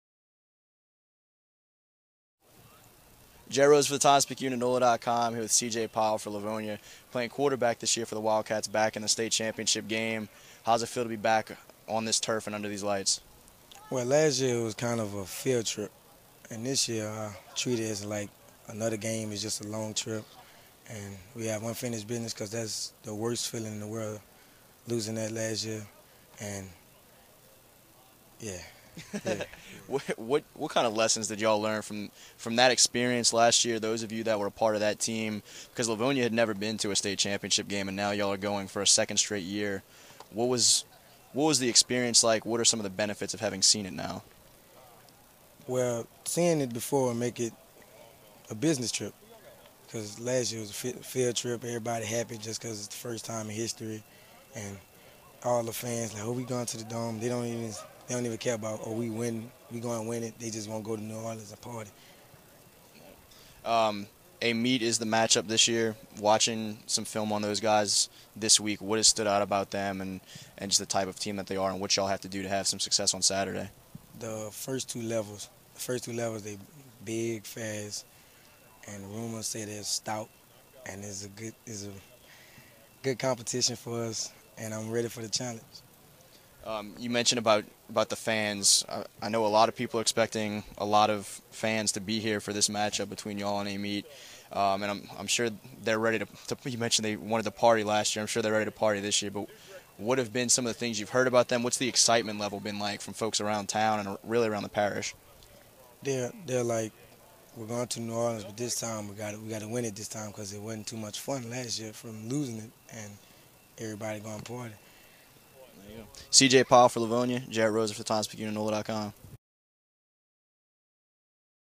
Livonia QB Cee Jay Powell talks about Saturday's Class 3A state title game
Livonia QB Cee Jay Powell talks about Saturday&apos;s Class 3A state title game against Acadiana